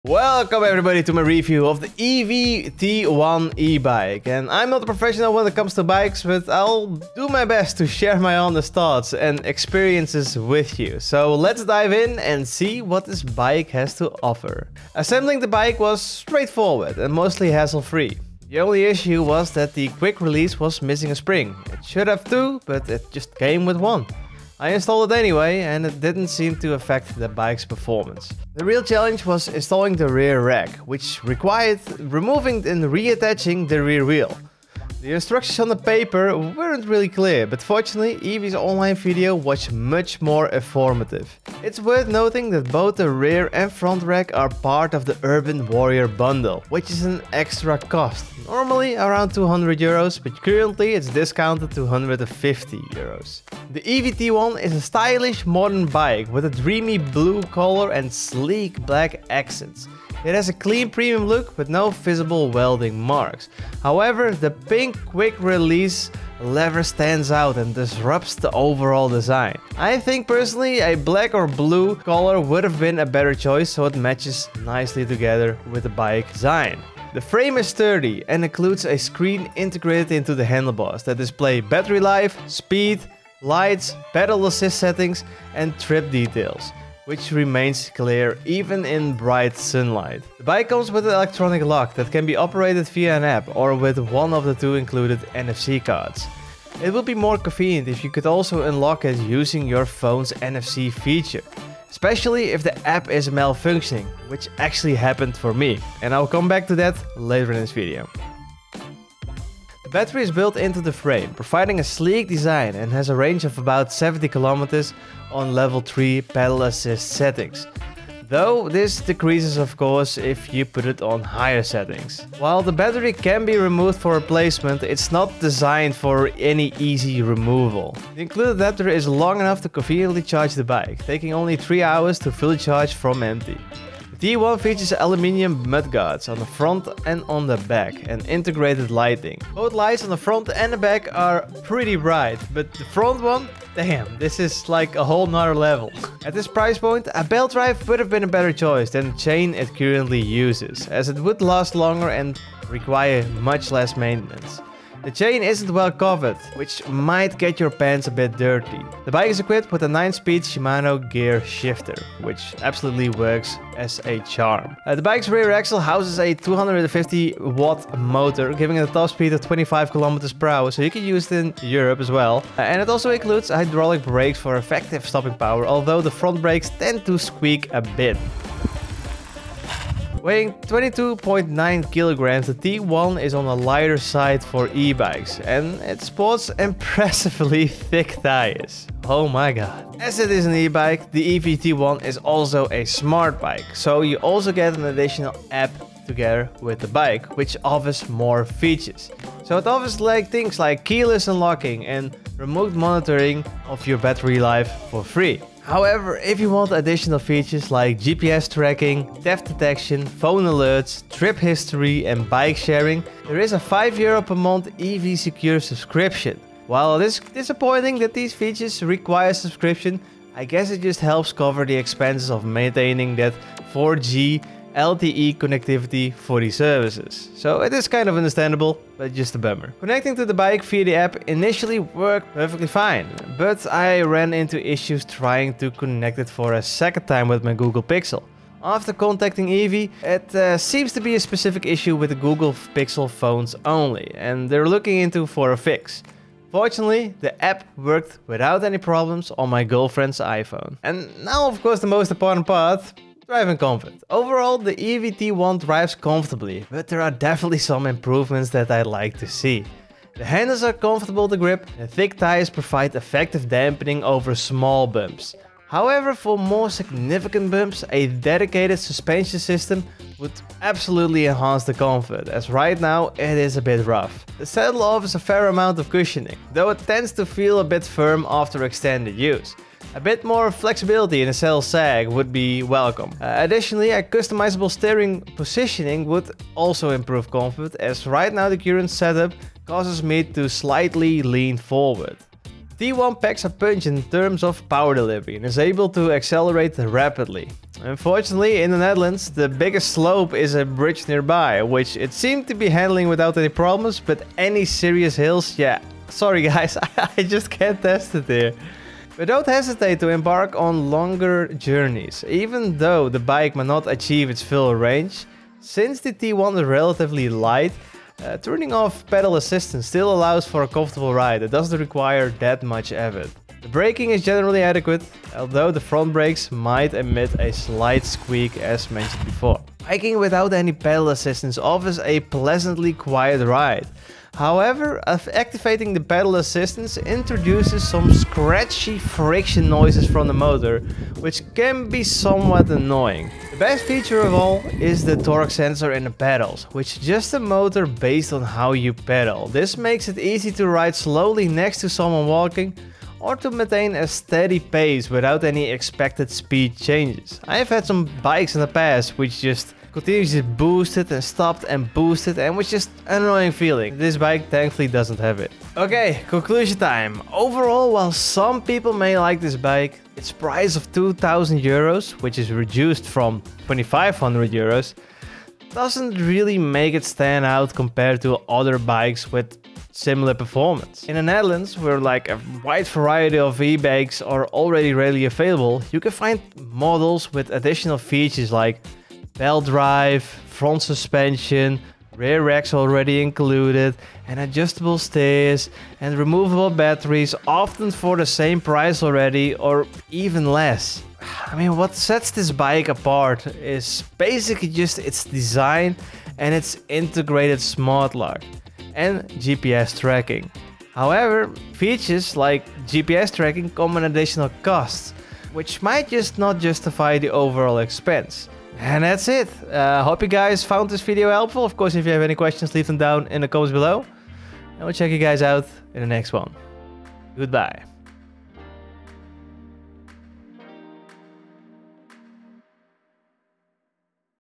The EVIE T1 has some nifty smart features, but it does come at a cost...
Today we are reviewing the Evie T1 E-Bike! The Evie T1 has a couple of nifty features up its sleeve that would make it really as a daily driver. But some of those also come at a cost.